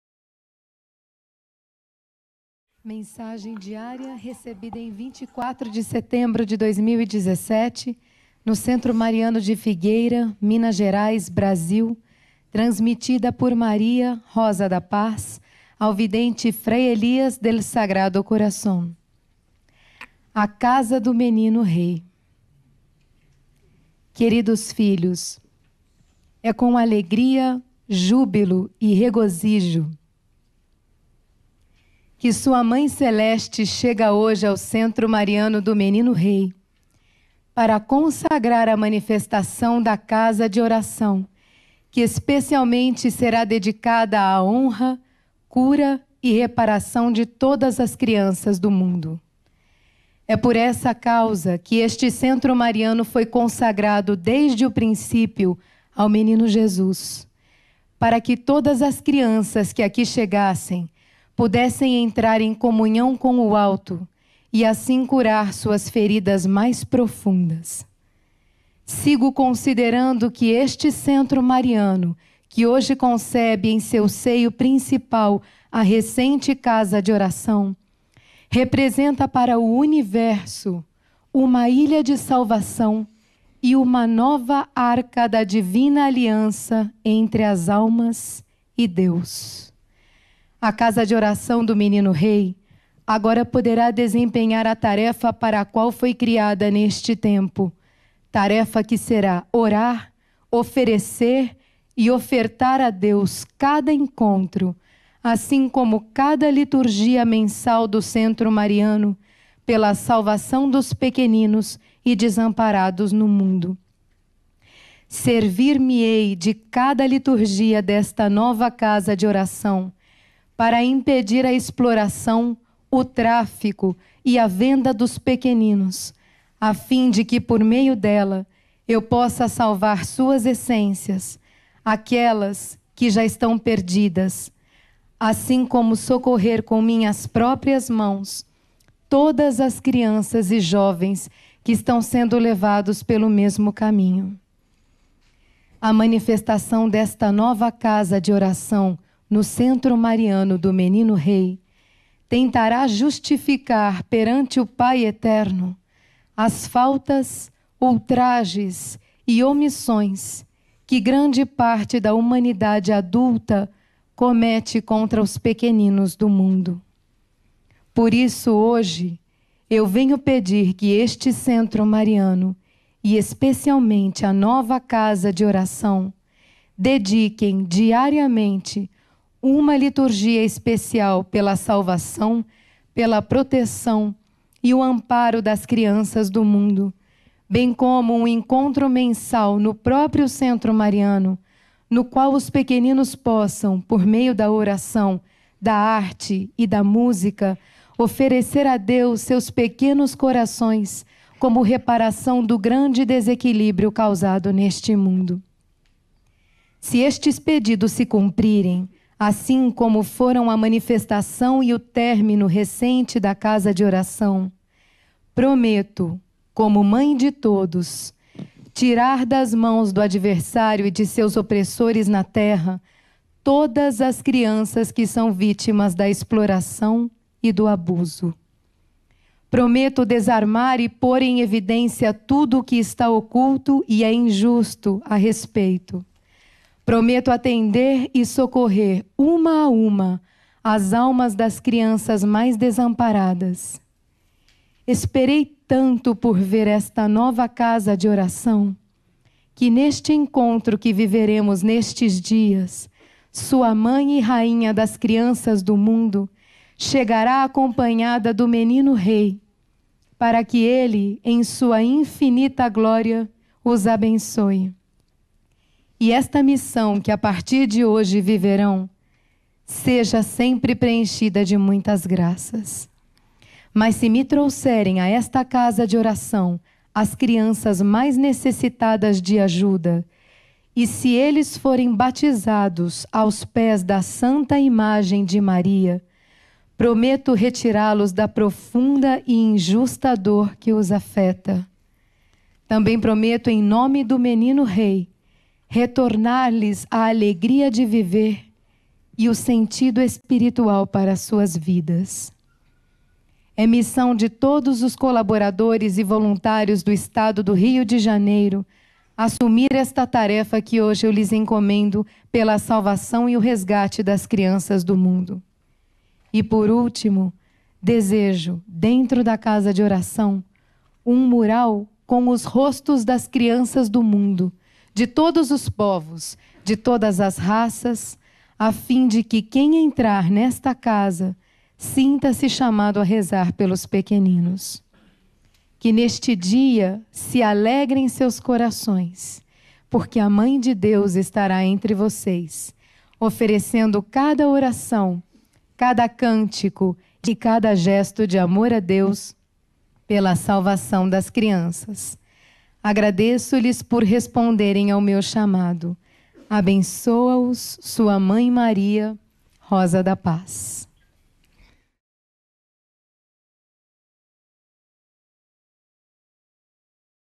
Message of the Virgin Mary - September 24, 2017 (Teresópolis, Brazil)
Vigil of Prayer for Peace in the Nations
Praying the Hail Mary in languages
Sunday, 24 of September
Marian Center of the Child King, Brazil